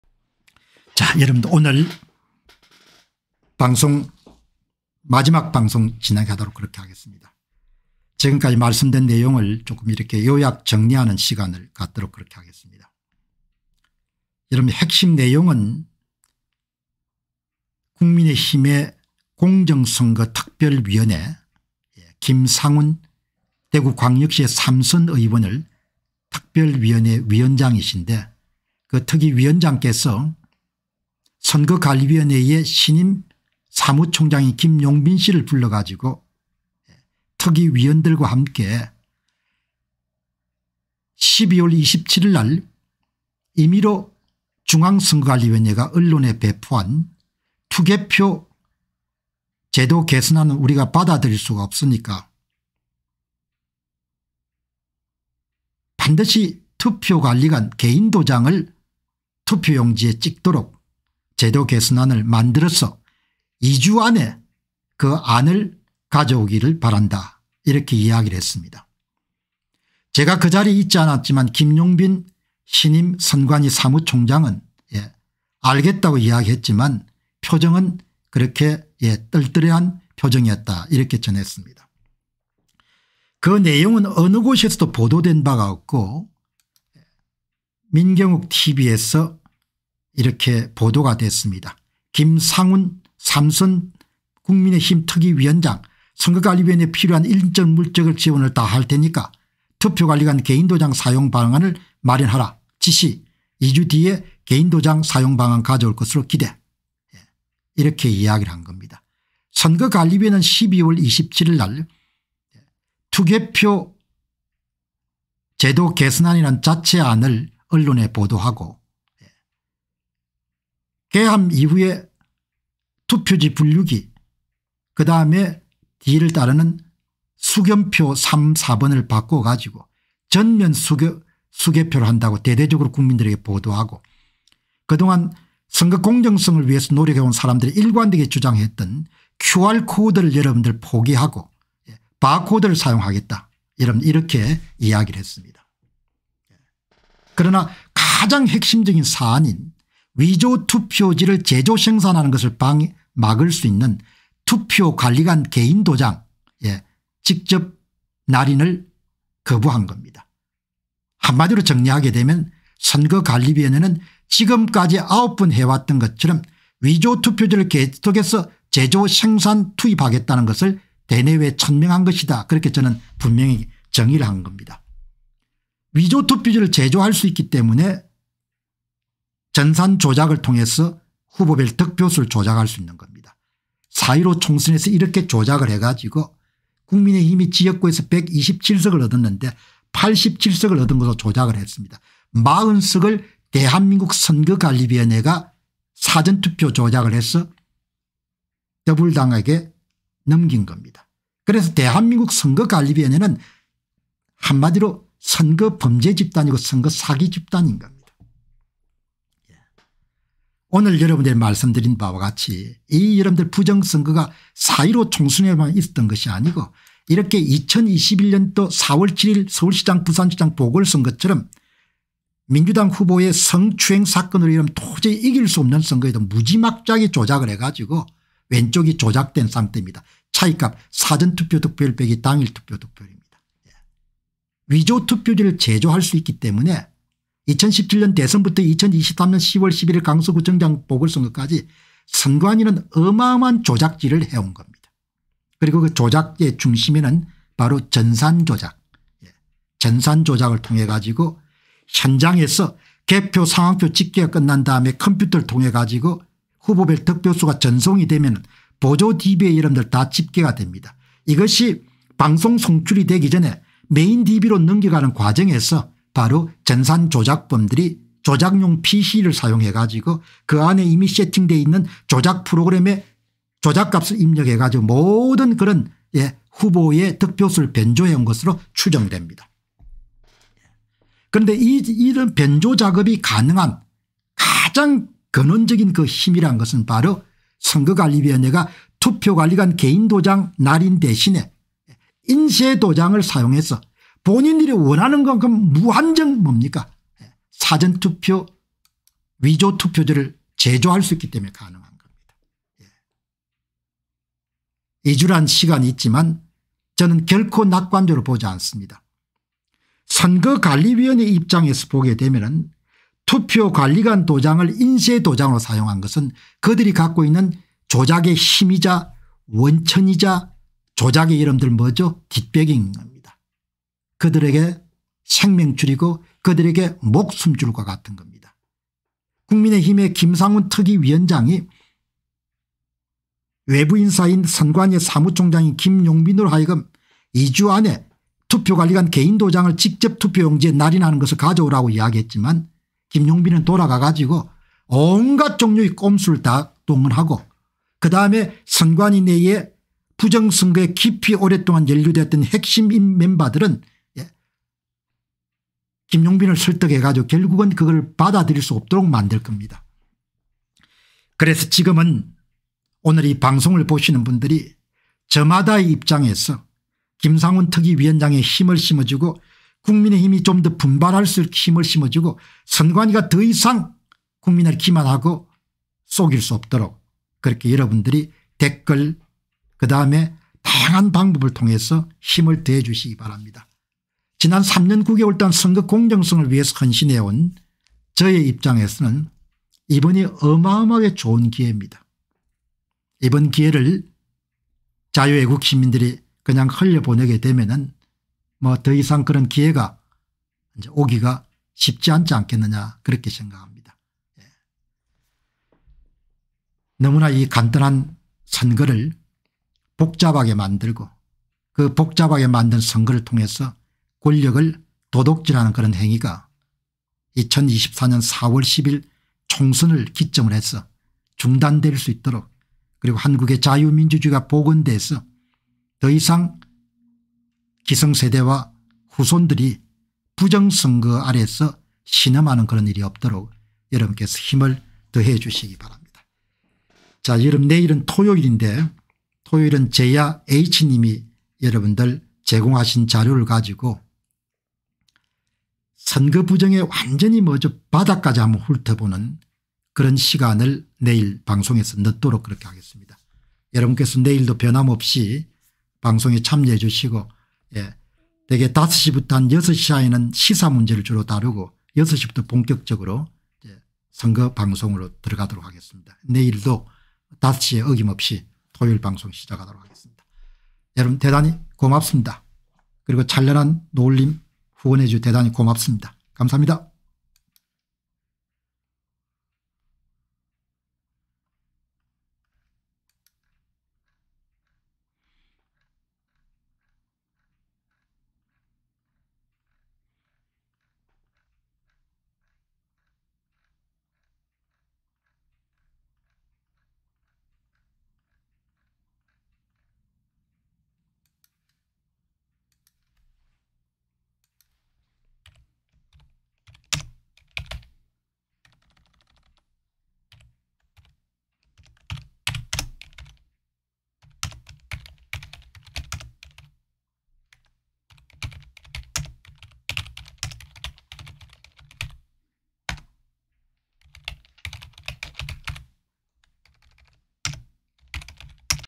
선관위, 인쇄도장 고집 / 국힘, "2주 안에 투표관리관 개인도장 날인 방안 마련하라" 강력 요구 / 선관위, 위조투표지 제조 필수적인 인쇄도장, 내놓지 않을 것 [공병호TV]
Bitcoin : 3MTW849AkP2ftLeSK2teZHMmzLu7Z83s9U